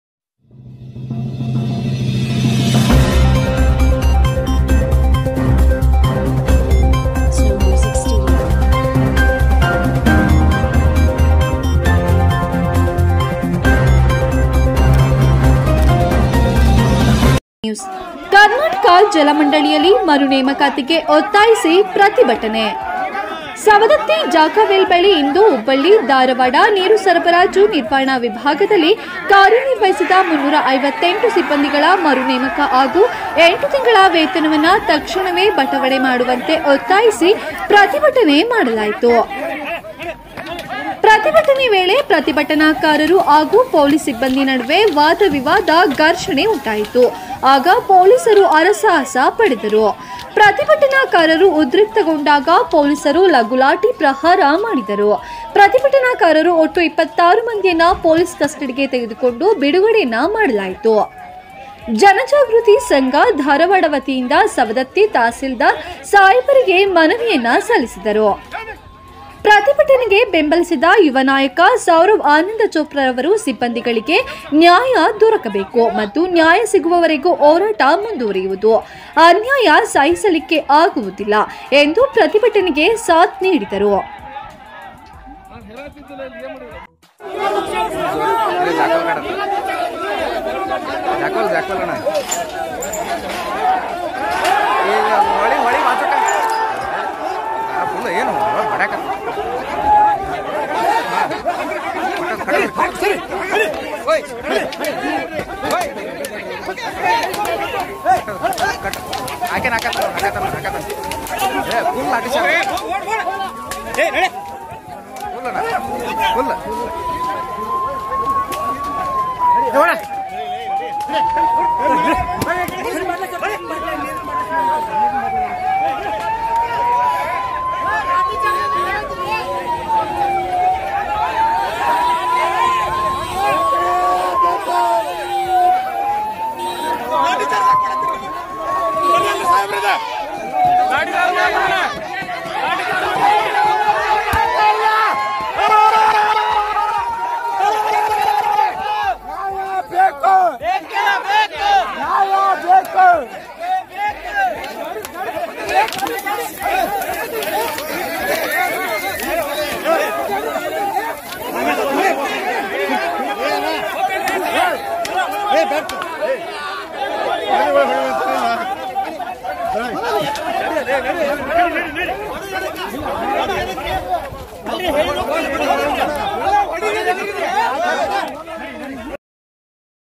SAUNDATTI|| ಕರ್ನಾಟಕ ಜಲ ಮಂಡಳಿಯಲ್ಲಿ ಮರು ನೇಮಕಾತಿಗೆ ಒತ್ತಾಯಿಸಿ ಪ್ರತಿಭಟಿಸಿದರು
SAUNDATTI BREAKING NEWS